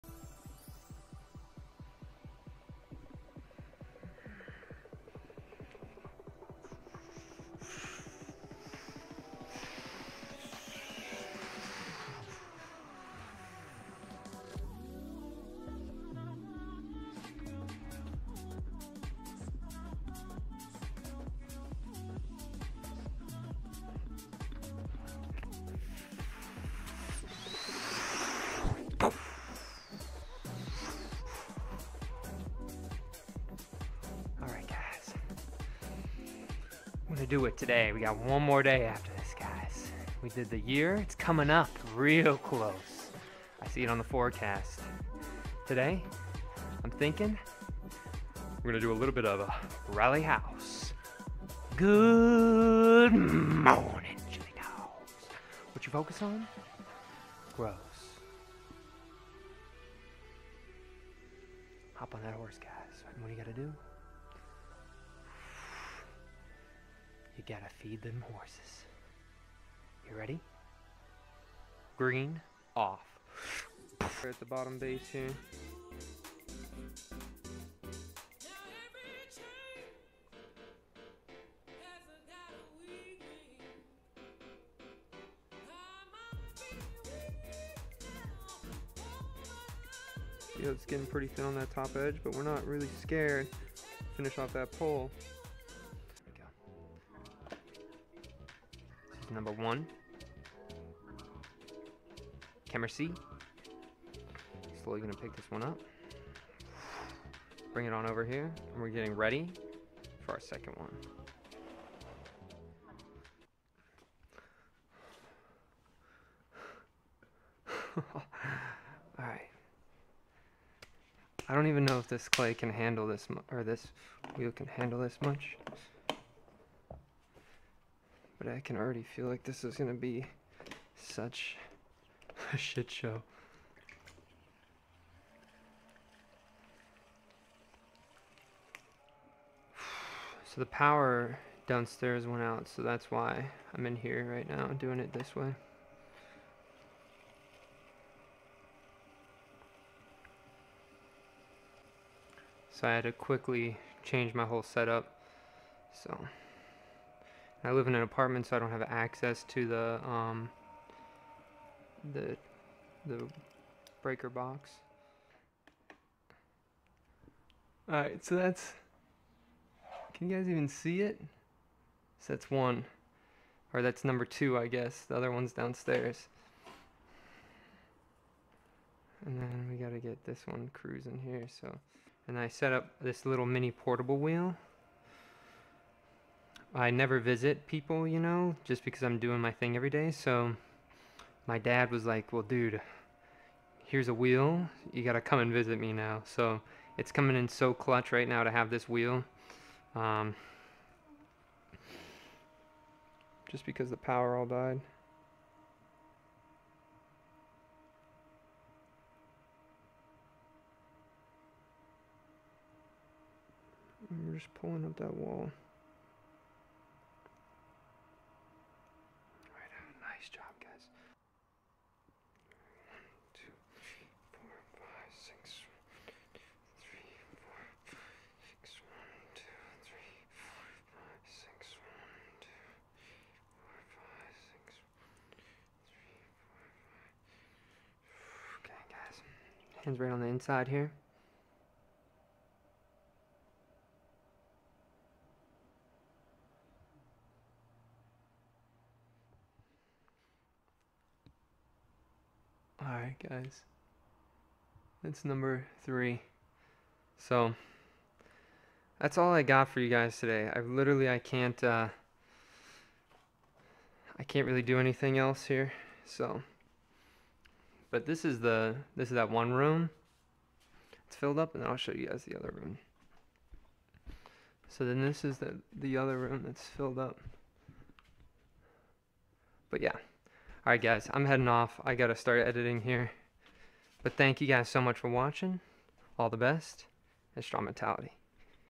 #289 Power Sabotage
1 day left to go and the power downstairs turned off. we adapted, and we get it done.